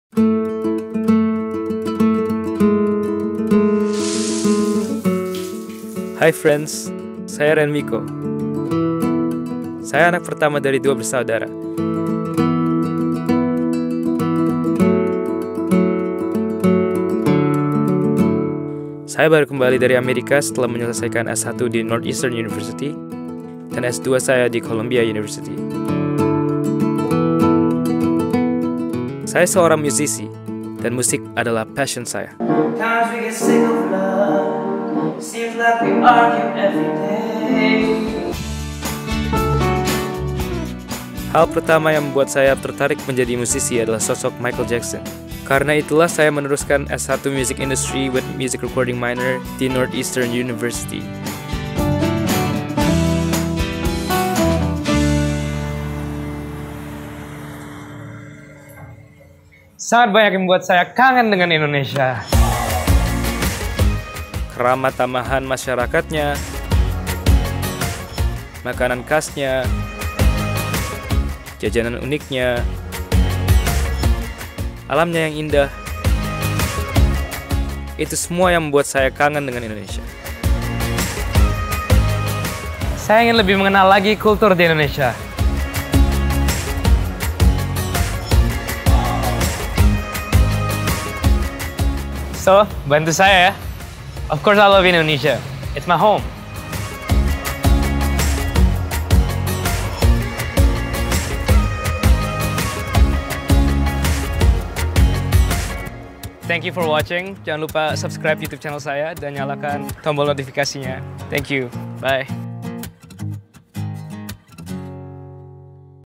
Renn Miko - Selamat Datang, Friends!
Hai, Friends! Saya Renn Miko, saya seorang penyanyi dan penulis lagu asal Indonesia, setelah lulus dari sekolah musik di Amerika ia memutuskan untuk merintis karir saya di Indonesia. Sangat banyak yg membuat saya kangen dengan Indonesia.

Jangan lupa nyalakan tombol lonceng notifikasinya untuk mendapatkan update konten terbaru dari Saya!